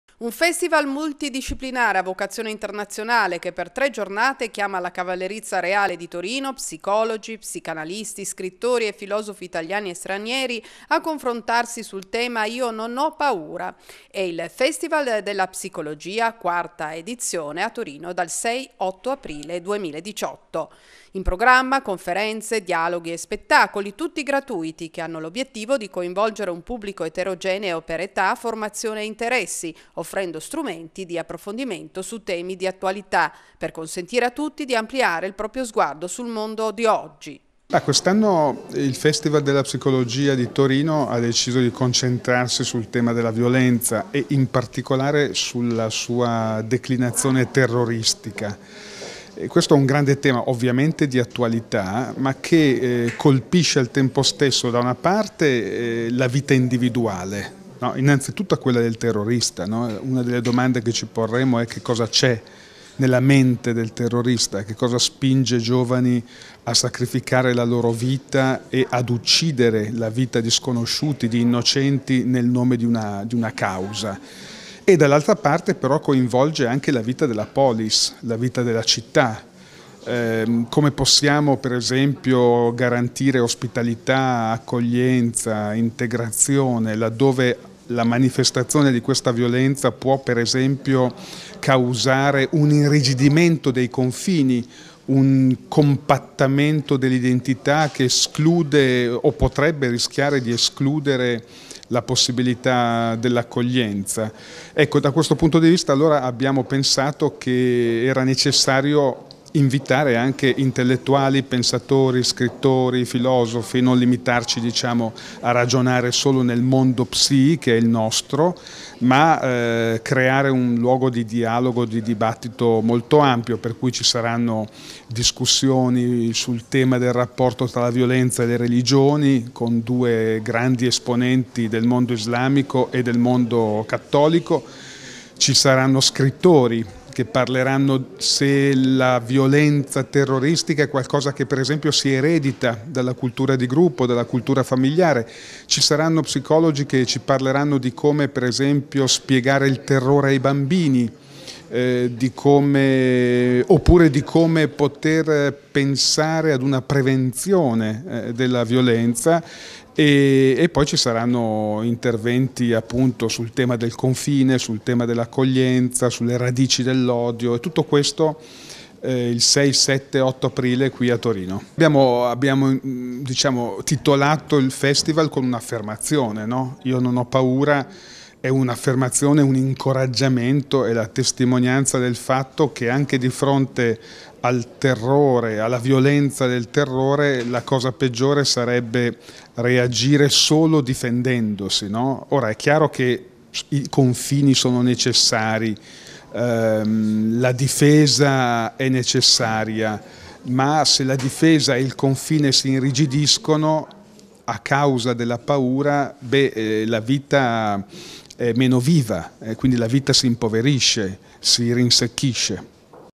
Primantenna 13 03 2018
Servizio di Primantenna girato da Paola Caramella alla conferenza stampa di martedì e andato in onda nel notiziario delle 19.30 di martedì e in replica ieri alle 13.30 e 19.30.